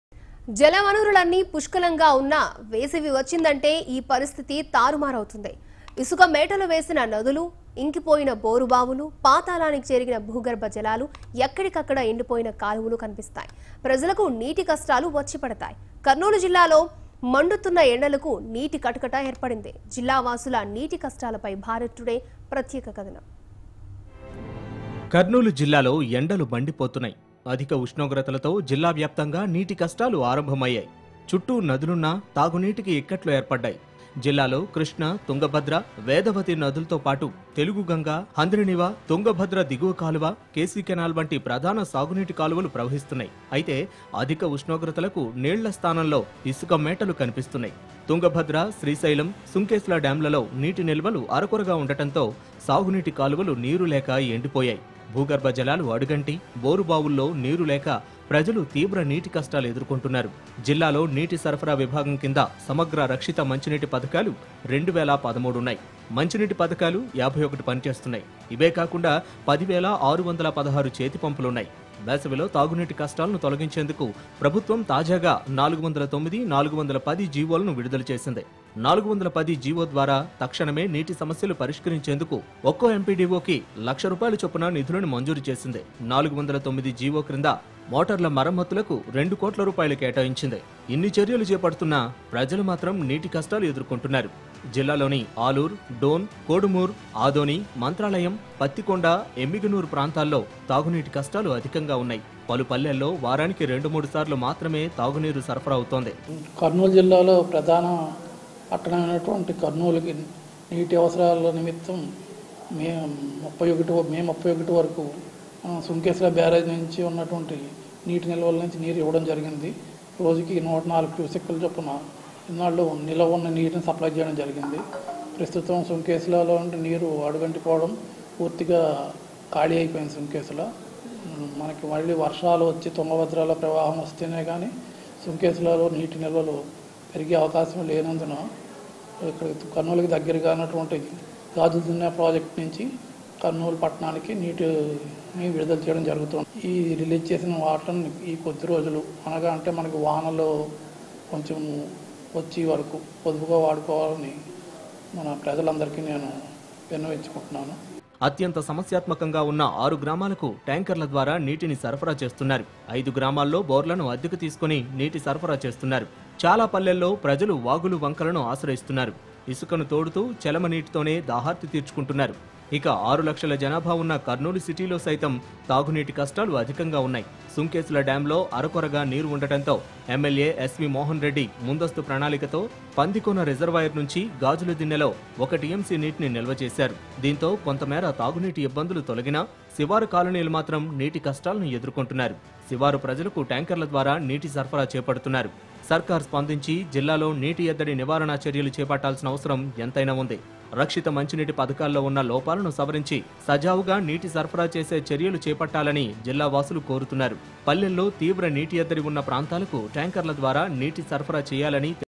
కర్నూల్ దాహం | Special Story On Summer Water Problem In Kurnool | Drinking Water Crisis
కర్నూల్ దాహం | Special Story On Summer Water Problem In Kurnool | Drinking Water Crisis | Bharat Today